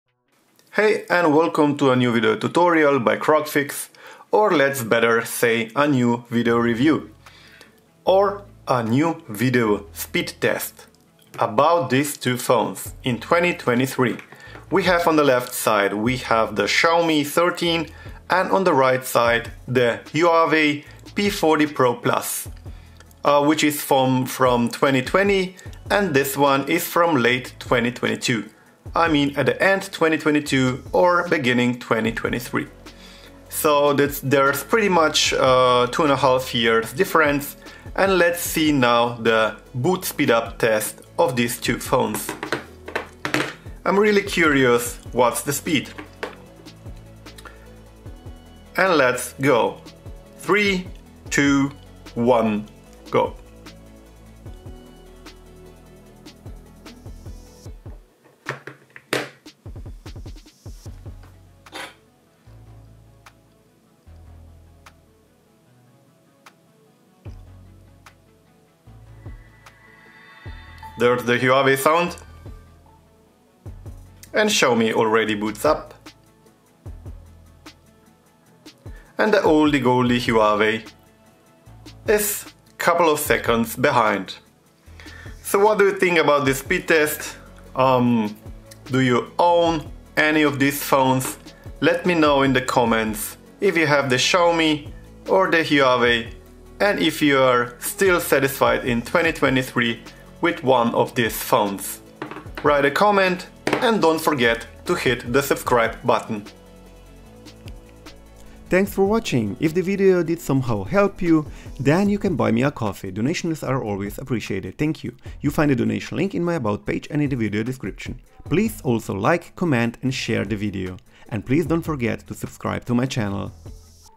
Xiaomi 13 vs Huawei P40 PRO+ / Boot Speed test - ready to use
Which one is faster when you start the phones? The XIAOMI 13 or the HUAWEI P40 PRO Plus? What do you think?

Find great DEALS on this two phones HERE:
As an eBay Partner, I may be compensated if you make a purchase through the links.

US & GLOBAL
Xiaomi 13
Huawei P40 Pro+

EUROPE
Xiaomi 13
Huawei P40 Pro+